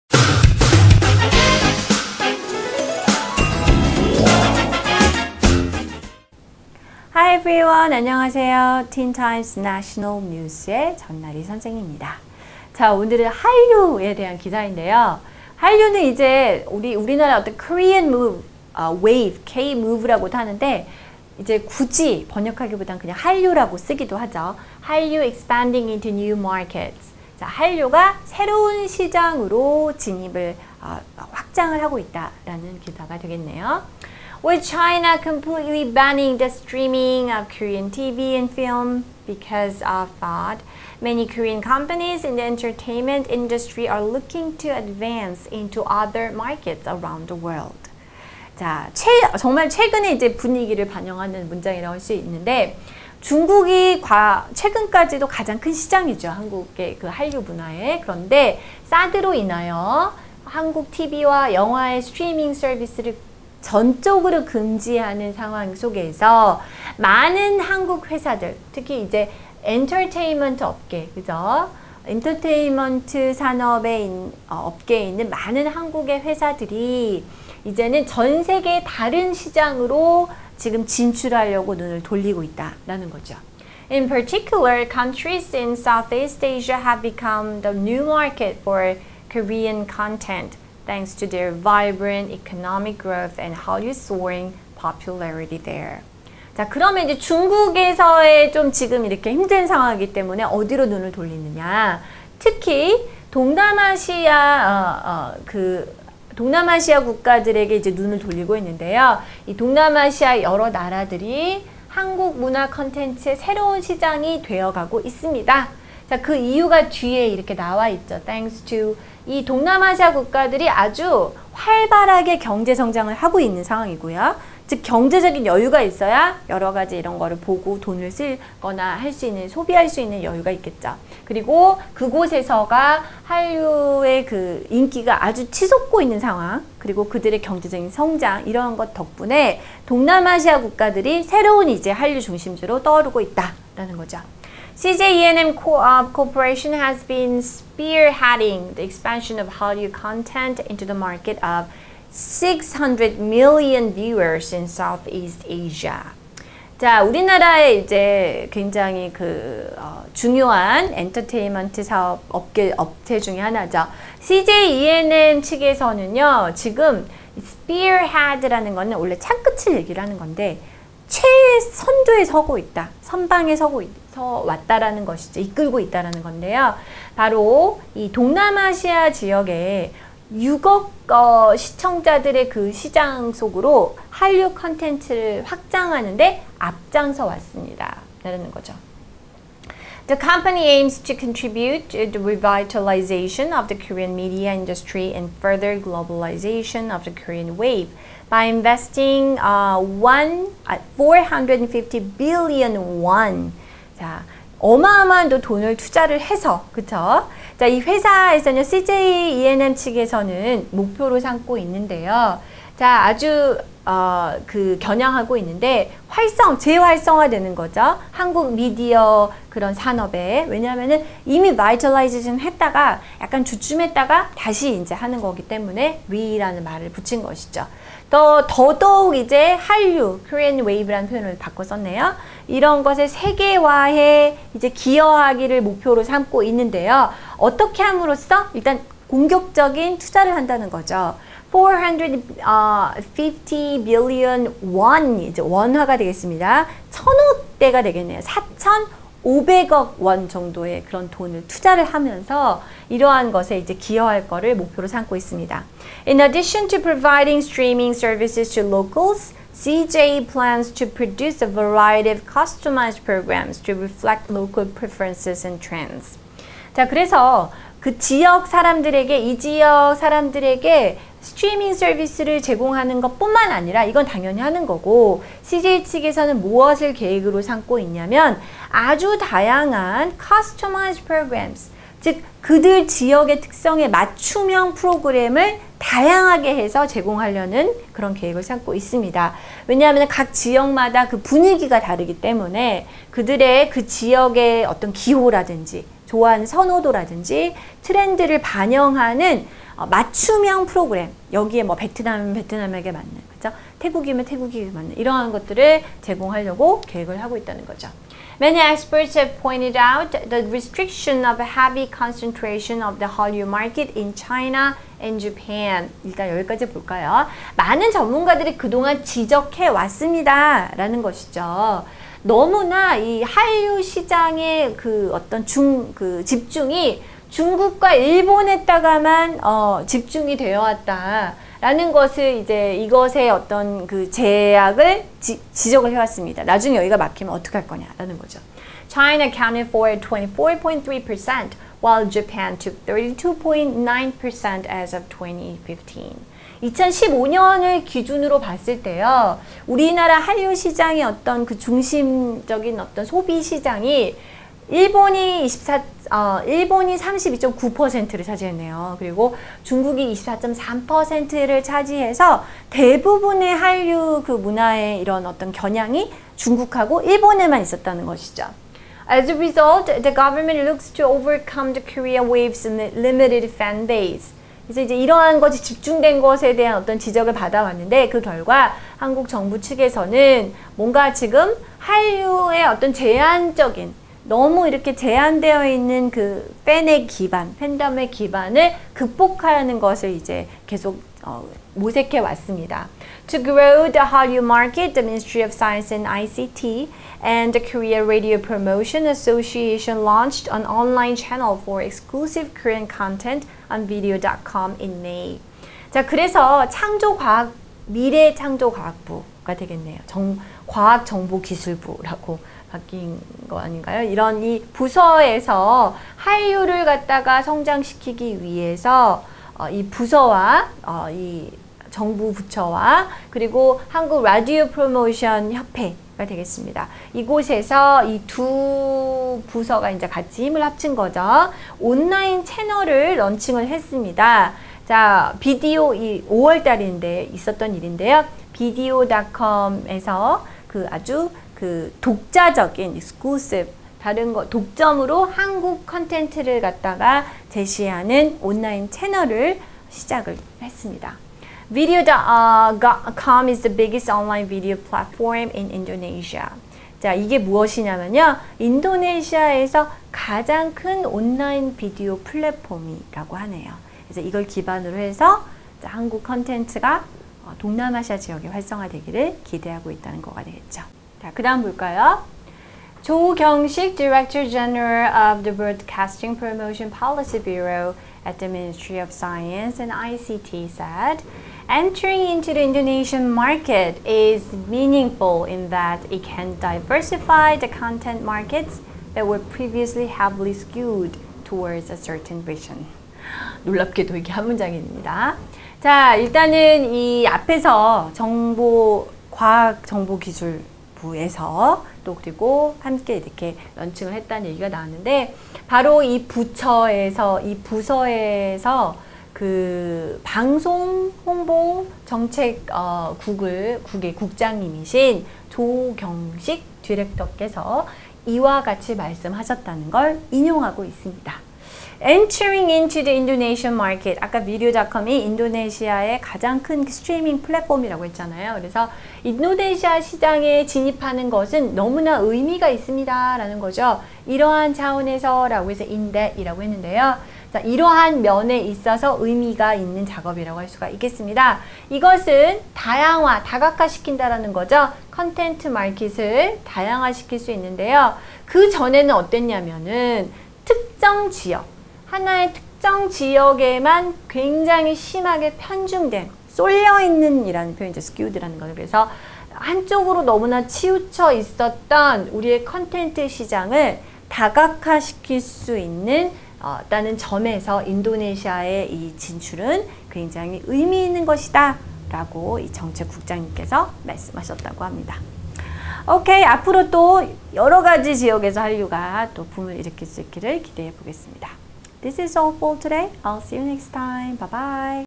Hallyu Expanding Into New... - 전나리 선생님과 함께 하는 National News I 동영상 강의 ( The Teen Times - No.843 )
청소년 영자신문 틴타임즈 843호 National News I 동영상 강의

새로운 시장 개척하는 한류